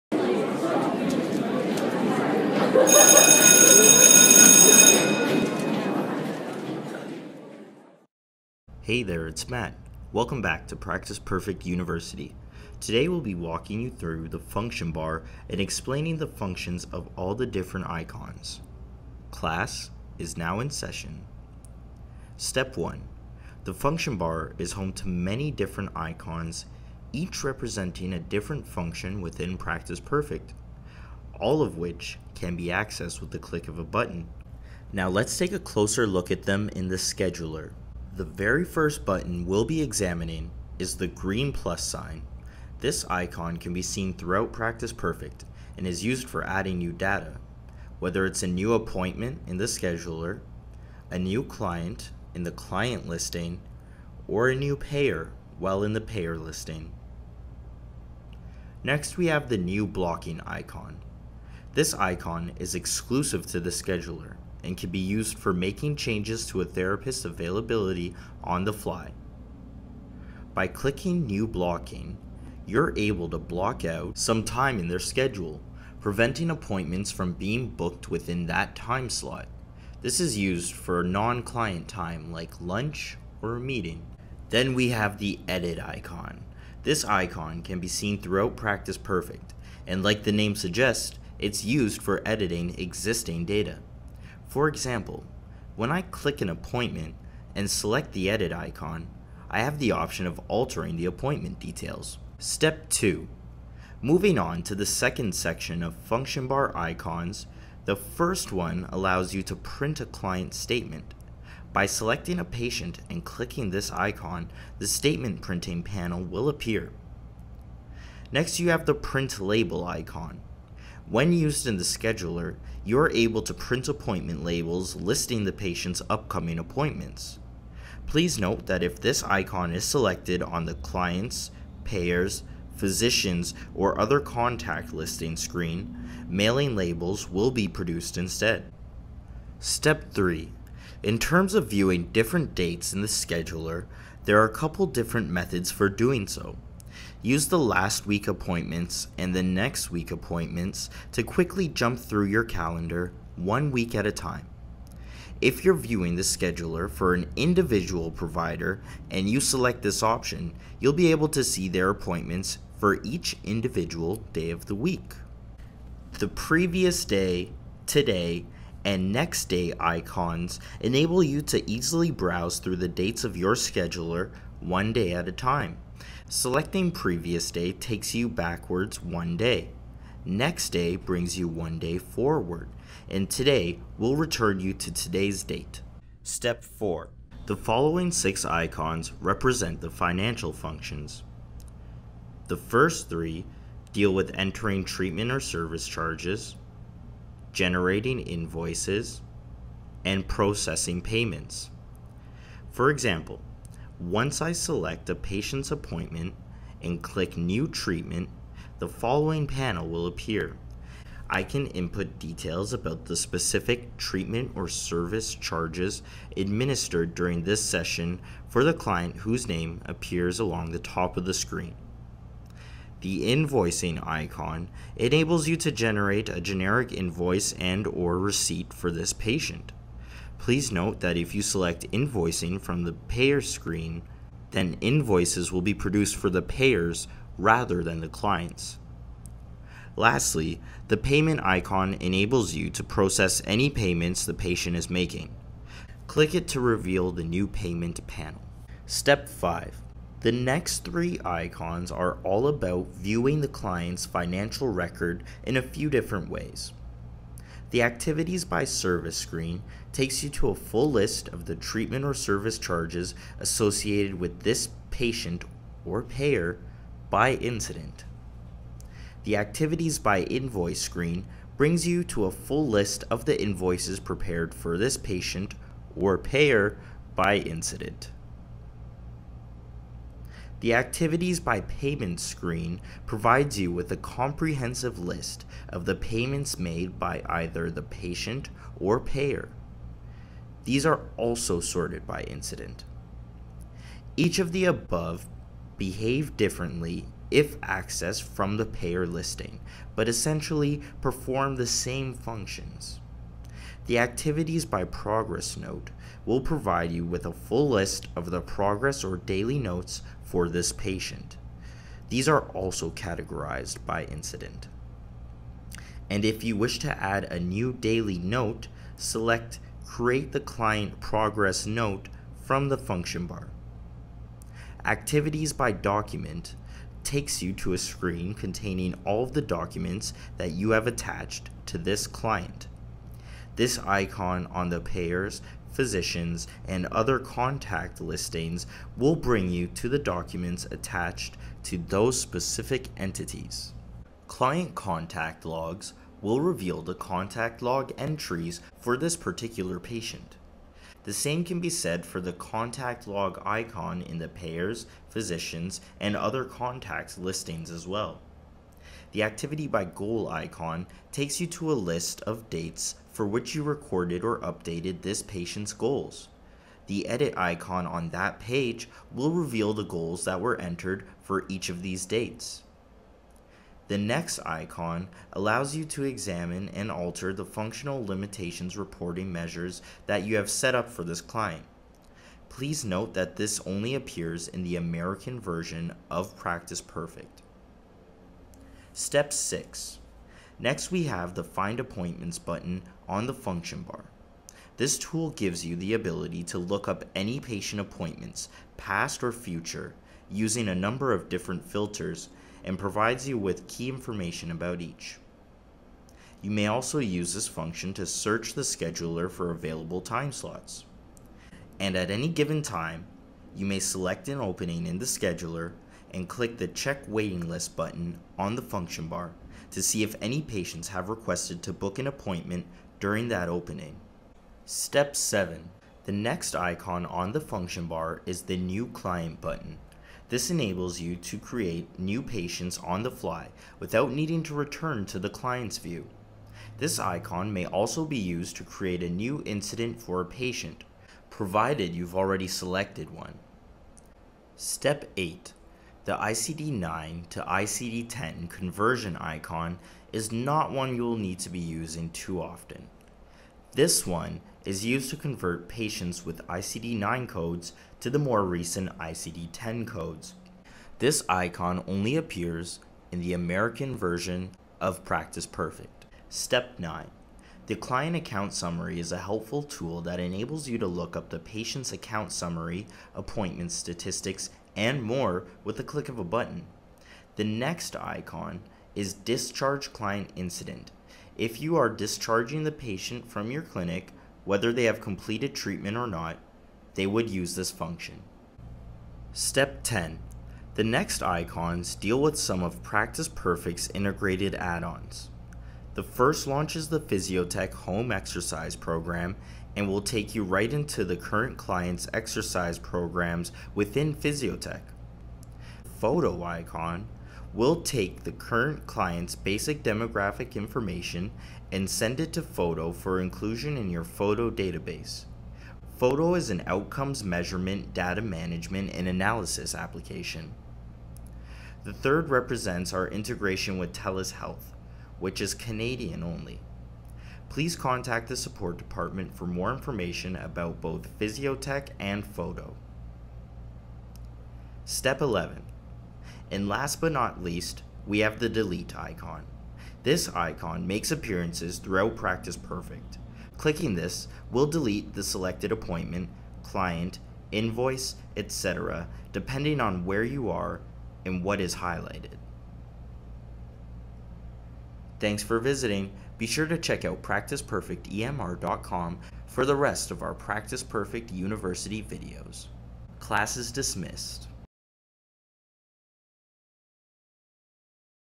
Intro to icons in Practice Perfect
This video tutorial explains the meaning behind each of the icons seen on the 'Function Bar'. While the specific icons discussed in this video are seen in the 'Scheduler', the icons discussed here apply throughout Practice Perfect.